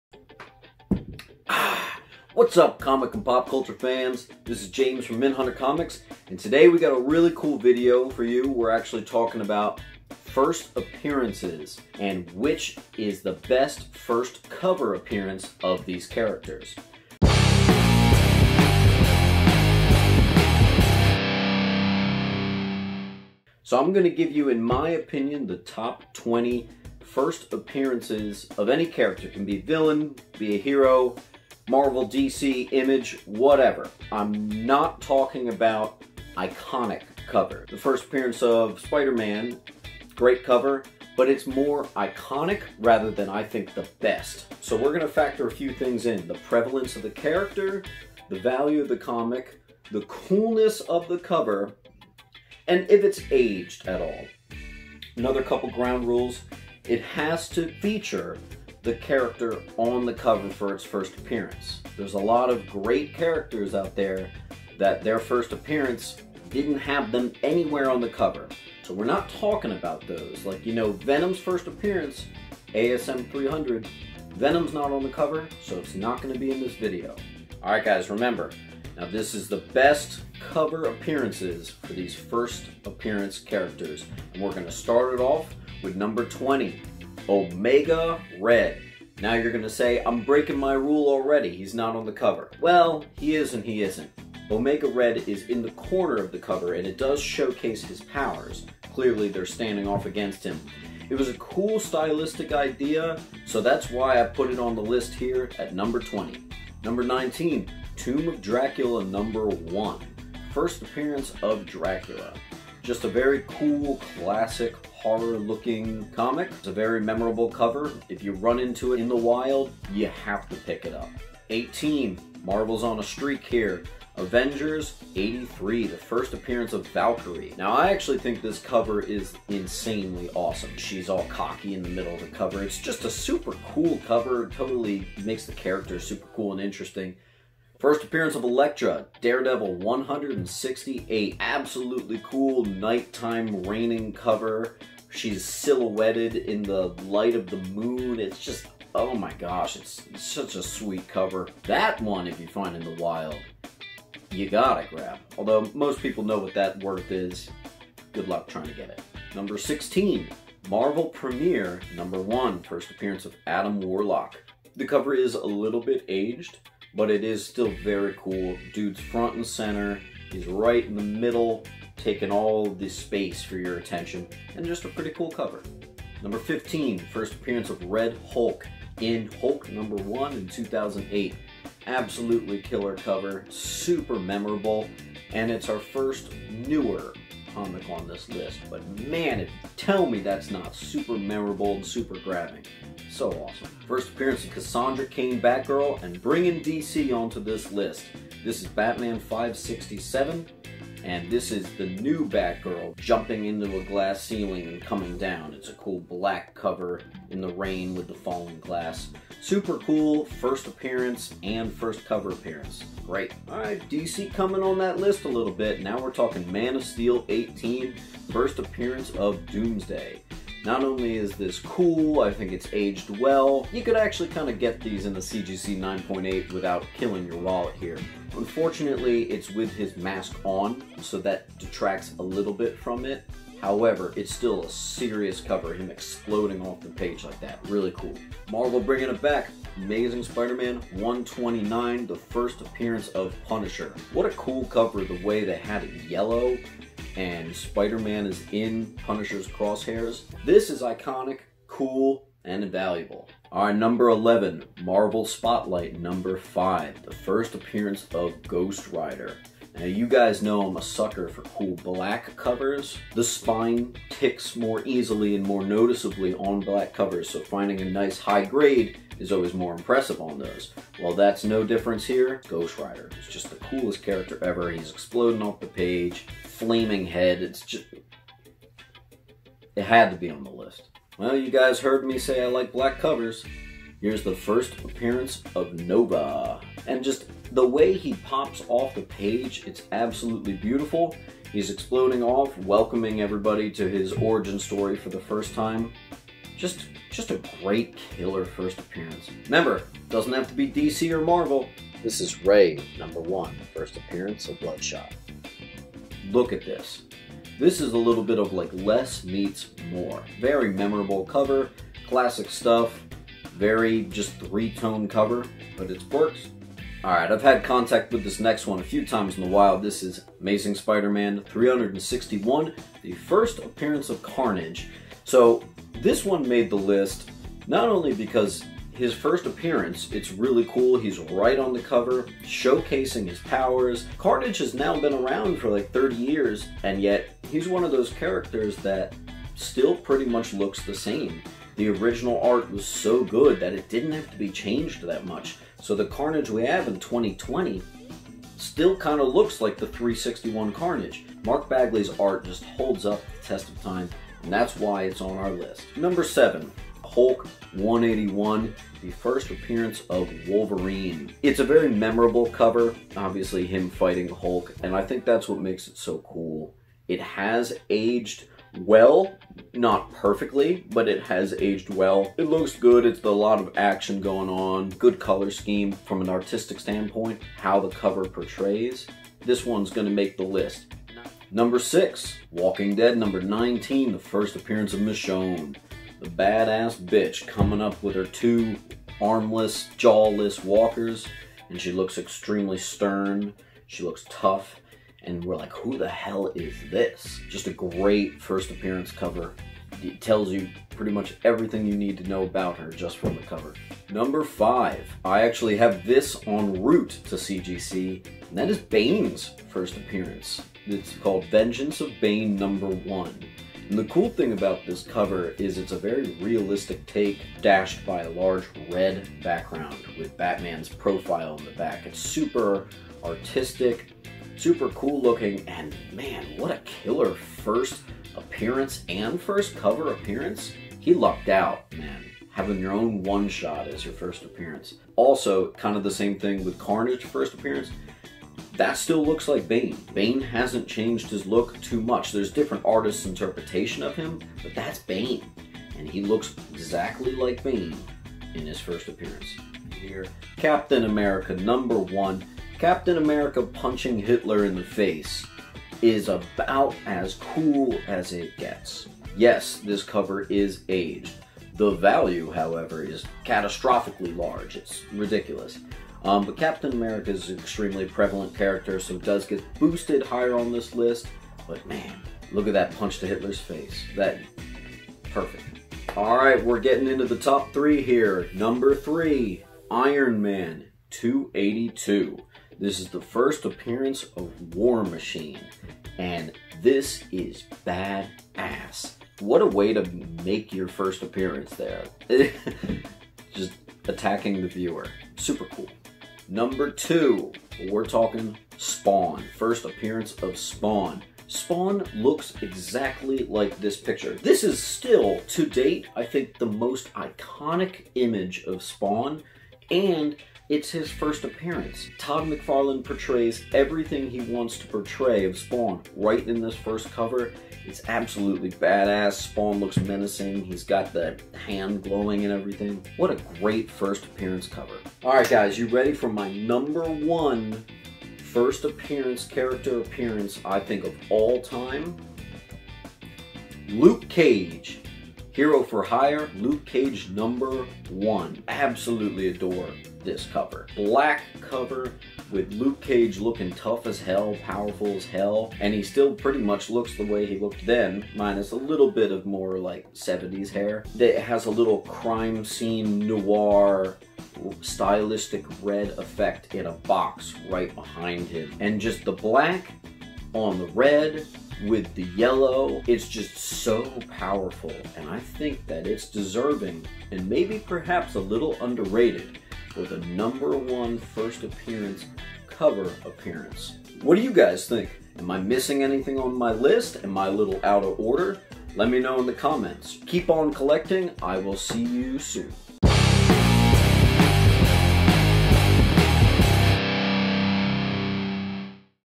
The TOP 20 First Appearance Comic Covers of All Time!
So many great characters are nowhere to be seen on the cover of their first issues, but what of the ones that are? Today I give you what I think are the best and coolest 1st appearance and cover appearances of Marvel, Dc, Image and more heroes and villains. This was such a fun video to make! We're not looking just at iconic covers but singularly dope covers in general. You'll see some books in here you may have forgotten about! Enjoy!